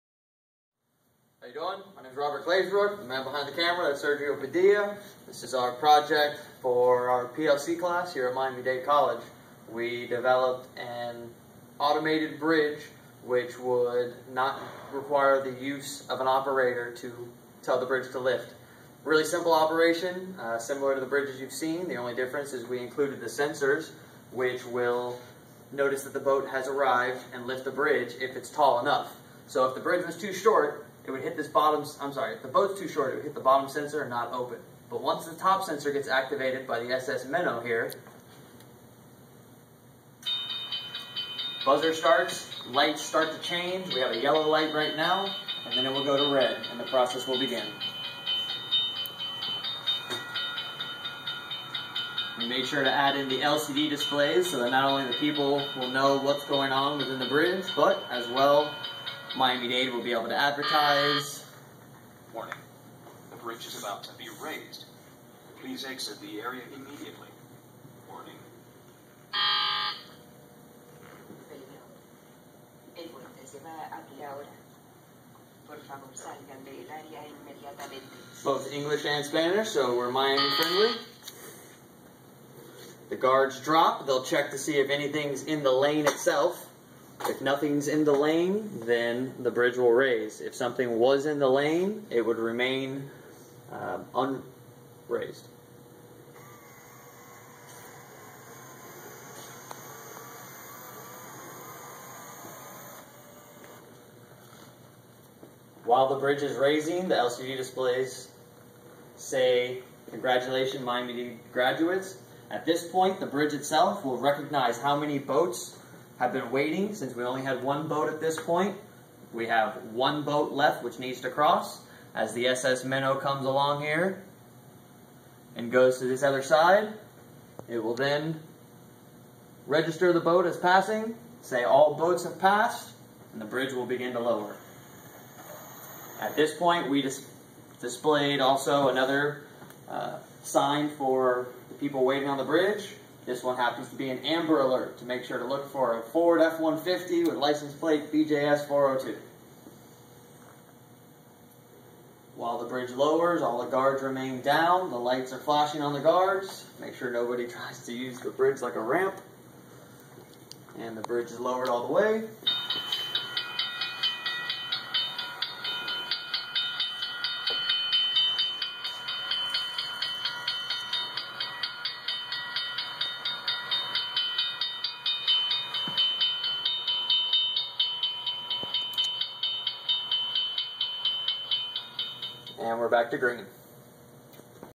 MDC autonomous drawbridge project part 1
This project was designed at Miami Dade College to demonstrate how a drawbridge can operate witbout human interaction. PLCs are used to control the operation of raising and lowering the bridge. It add